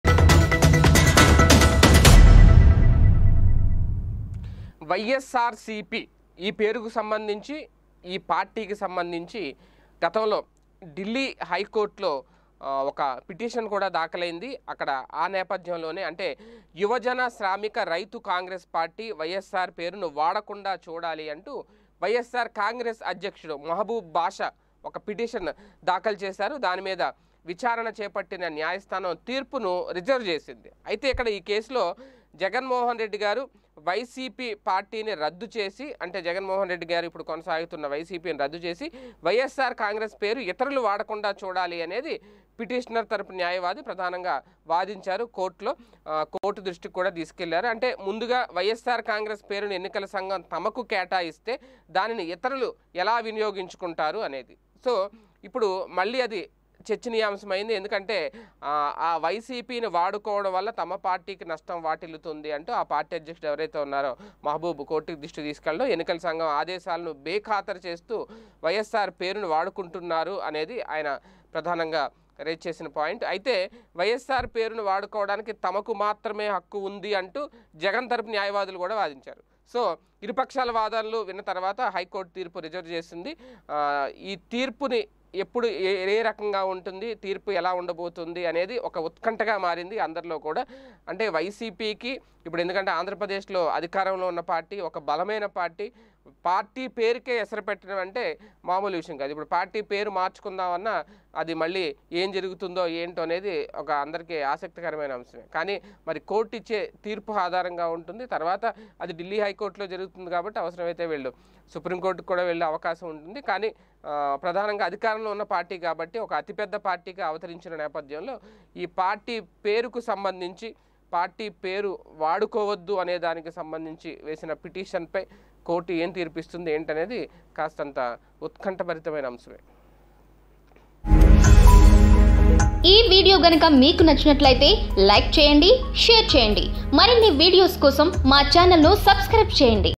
YCP Name Change..Petition in High Court|Ys Jagan|వైసీపీ పేరు మార్పు.. హైకోర్టులో పిటిషన్|Chandrababu
వైసీపీ పేరు మార్పు.. హైకోర్టులో పిటిషన్